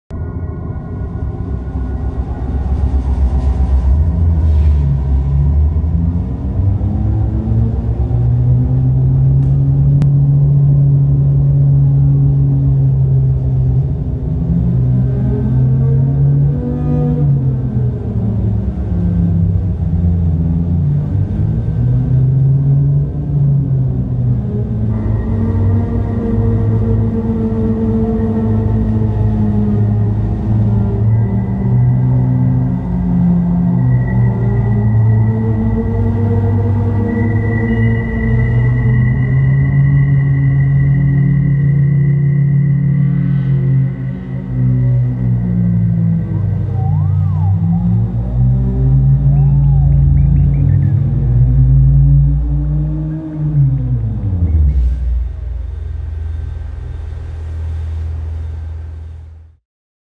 Intersection (interactive installation)
Intersection is an interactive installation based on 3 interconnected elements to create a mesmerizing effect: laser, projection and movement sensor.

Leap motion will be used to direct a laser towards the projection, which reacts to the movement of the spectator's hands.

While the installation acts as a reflection of the interaction of an individual in verse to society, it can be used for festivals, branding activation, malls or cultural centers.